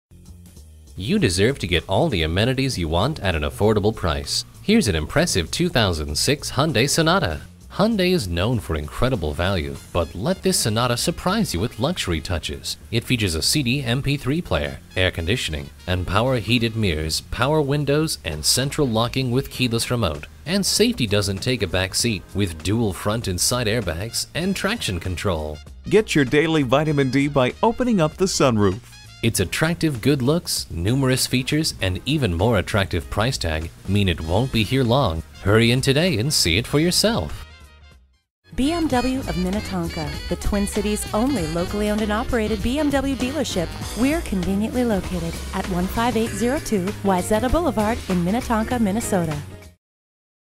2006 Hyundai Sonata Minnetonka MN Minneapolis, MN D80020A - SOLD
Year: 2006
Make: Hyundai
Model: Sonata
Trim: 4dr Sdn GLS V6 Auto 3.3L V6
Engine: 3.3 L V6 Cyl.
Transmission: 5-Speed A/T
Color: Steel Gray
Interior: Gray
Mileage: 88075
Stock #: D80020A
VIN: 5NPEU46F06H143619
Black Friday Hang Tag Specials Huge Savings Friday and Saturday Only!

Address:
15802 Wayzata Blvd